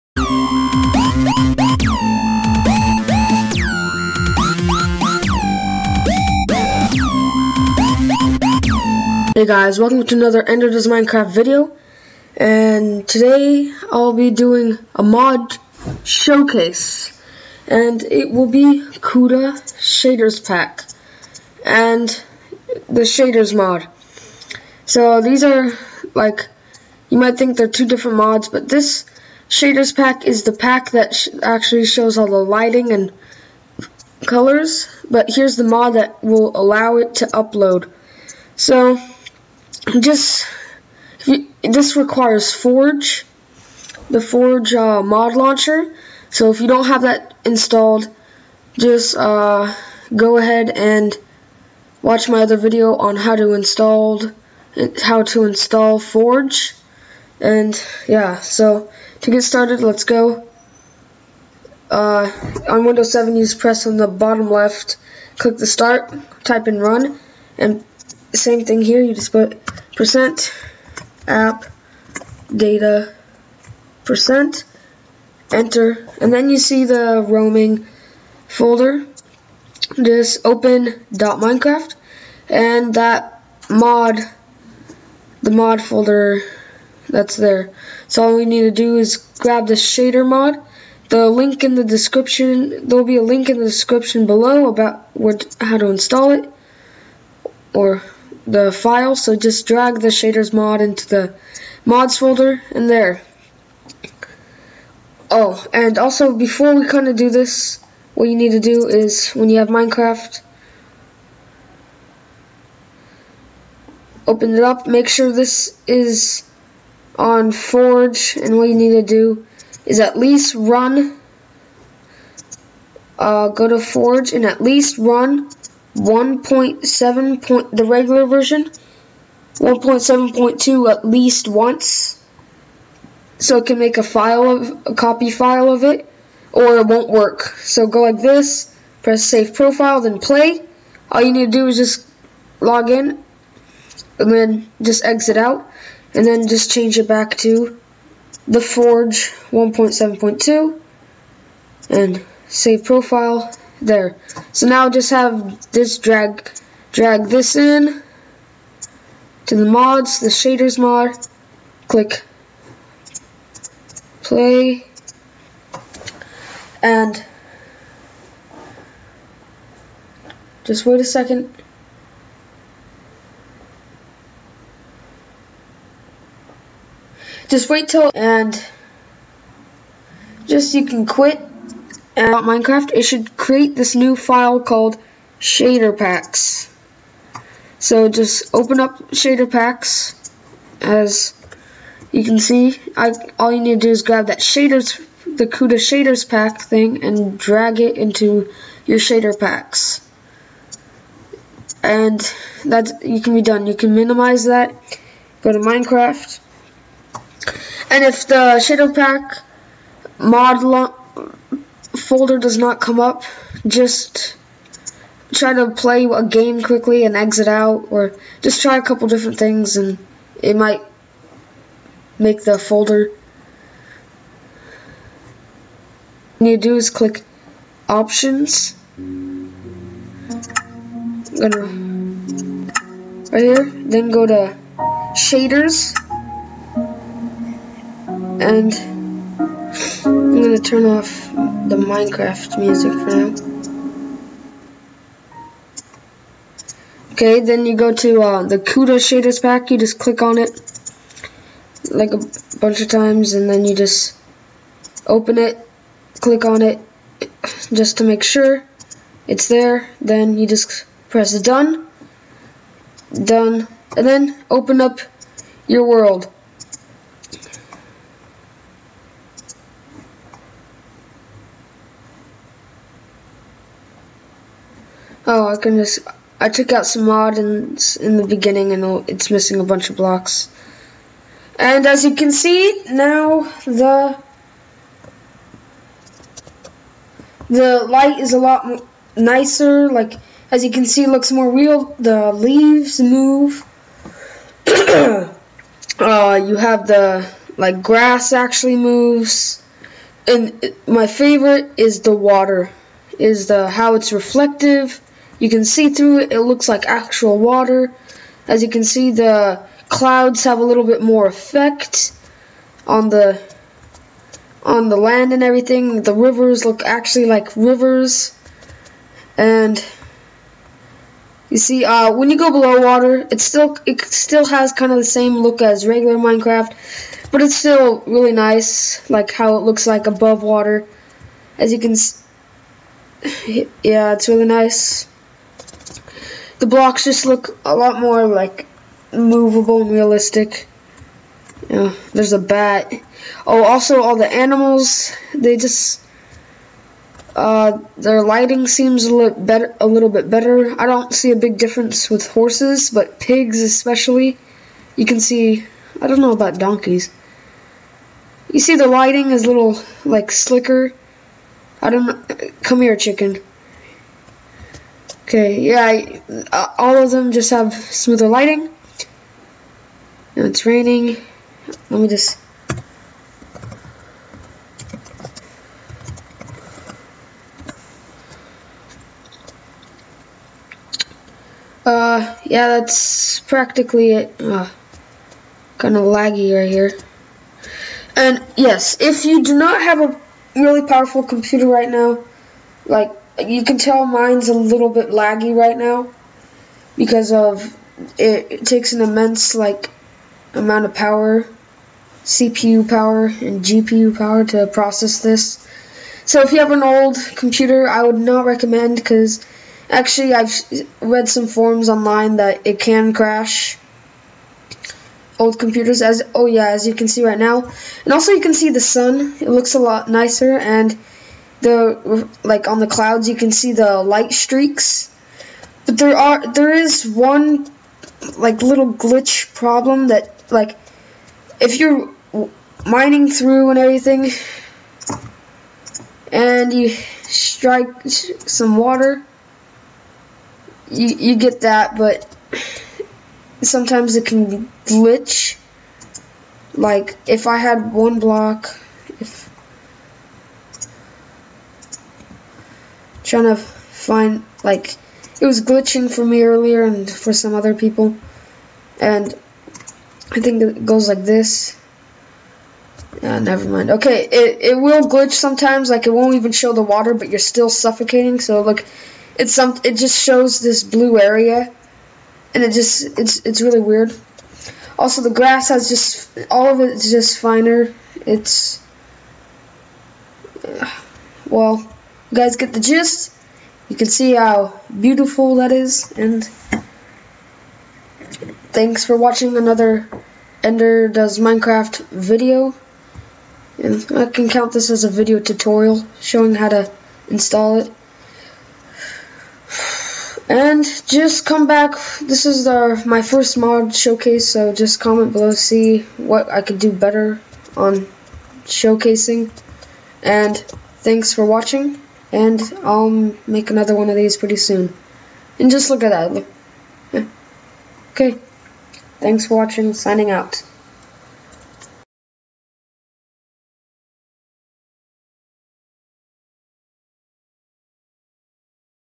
CUDA shader pack mod show case + Download
This is my first mod showcase but isn't this shader pack awesome!

If the CUDA SHADER PACK file doesn't come automatically into a zipped file right click on the file(Make sure you have 7zip or winrar) and press add to archive then click .zip then click OK.